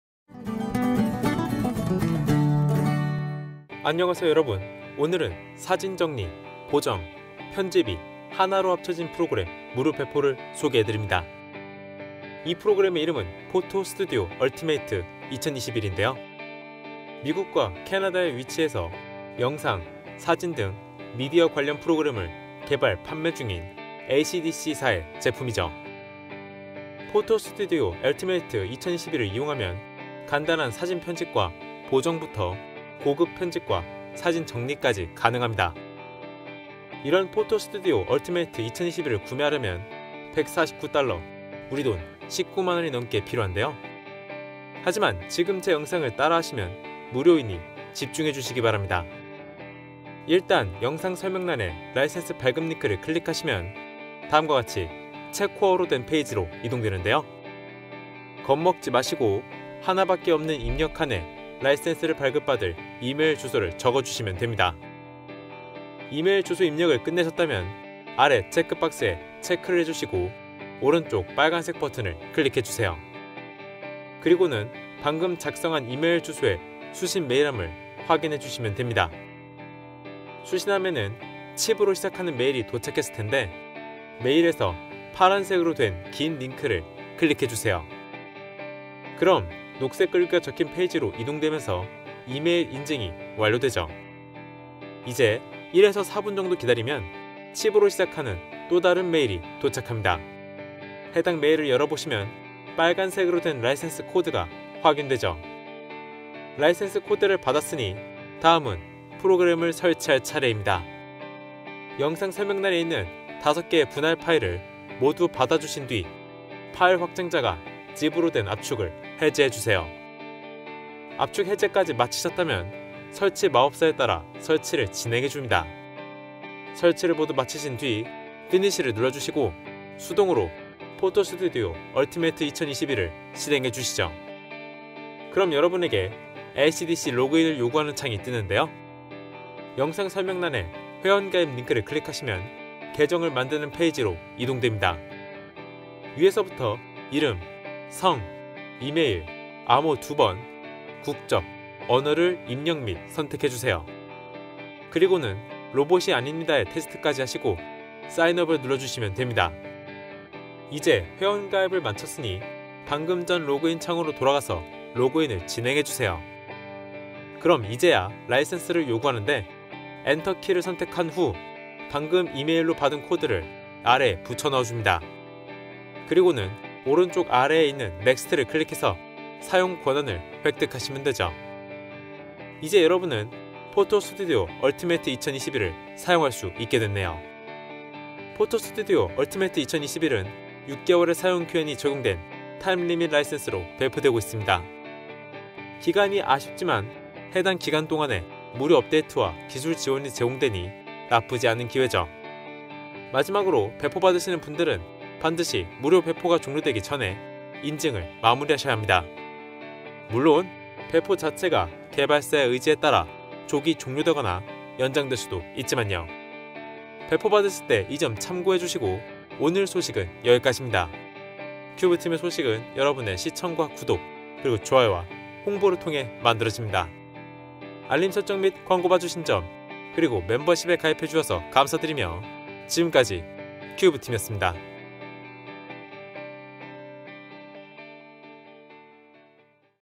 무료 소식 0564 # 만능 사진 정리 + 보정 + 편집 프로그램, Photo Studio Ultimate 2021 무료배포 (정가 190,000원)
* 사진 관리, 보정, 편집 모두 가능한 프로그램, Photo Studio Ultimate 2021 by ACDSee
* 정가 190,000원

- 배포 종료 : ?
※ 권리자의 의사에 따라 조기 종료 및 연장될 수 있으니, 명시된 기간이 끝났어도 시도해보시는 것을 추천드립니다.

※ CUBE-TEAM에게 한달에 한번 [컵라면/핫바/에너지드링크]를 사주고 싶다면!

* 참고 영상

* 예시 영상

* 사용된 소프트웨어 : Premiere pro CC 2020, Power point 365, Vegas pro 17 suite, Media Encoder CC 2020, MOVAVI Slideshow maker 2020